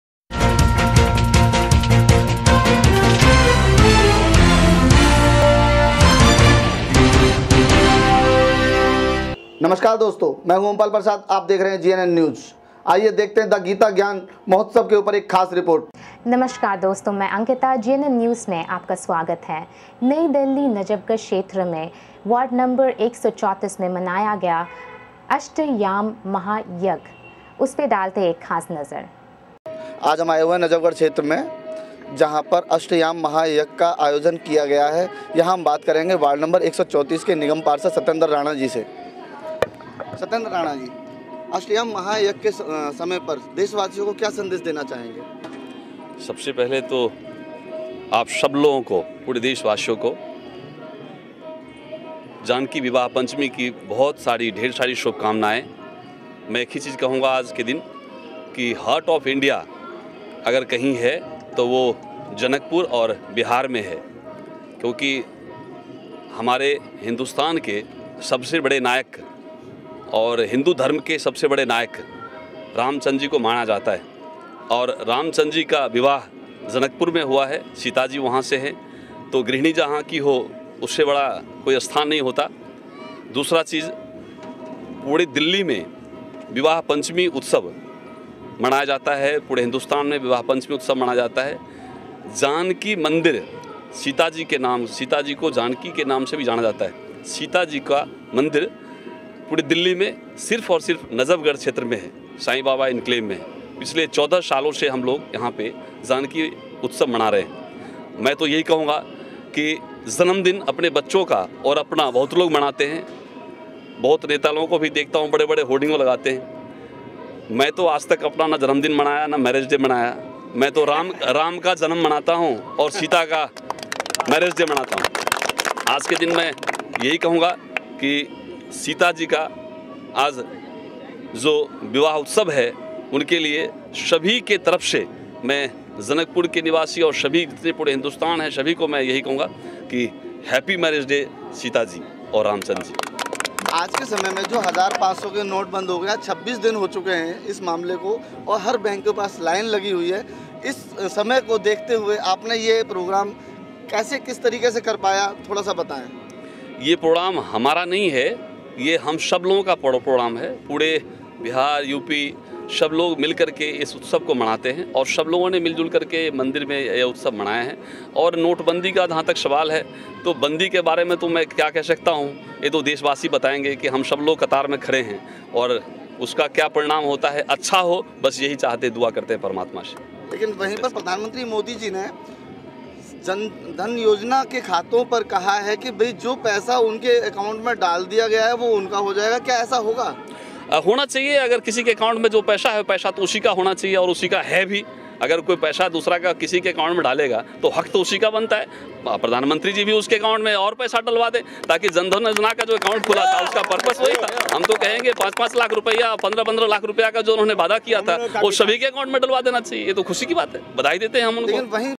GNN NEWS AGENCY: Nangli Ward N0.134 Ram Sita vivah or Vivah Panchami is the marriage anniversary
GNN NEWS AGENCY:EXCLUSIVE Nangli Ward N0.134 Ram Sita vivah or Vivah Panchami is the marriage anniversary of Lord Rama and Goddess Sita